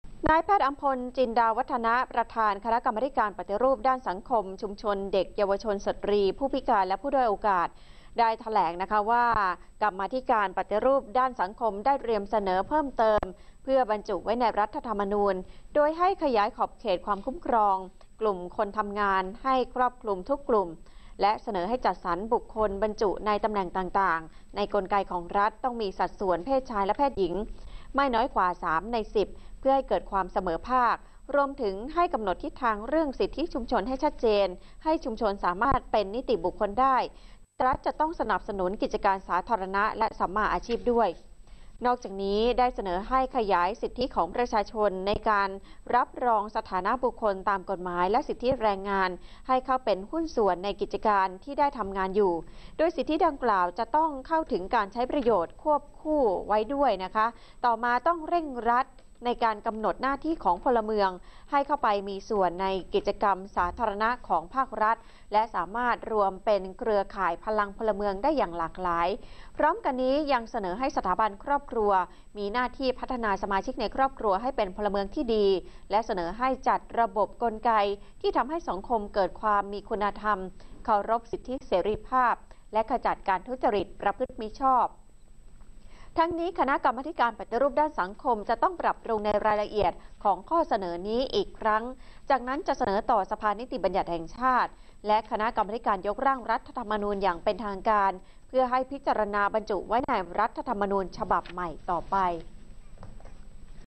กมธ.ปฏิรูปสังคม ชง 7 ข้อบรรจุ รธน.-กกต.นัดถกปฏิรูปการเมือง-เลือกตั้ง พรุ่งนี้
2014/12/1 กมธ.ปฏิรูปสังคม ชง 7 ข้อบรรจุ รธน.-กกต.นัดถกปฏิรูปการเมือง-เลือกตั้ง พรุ่งนี้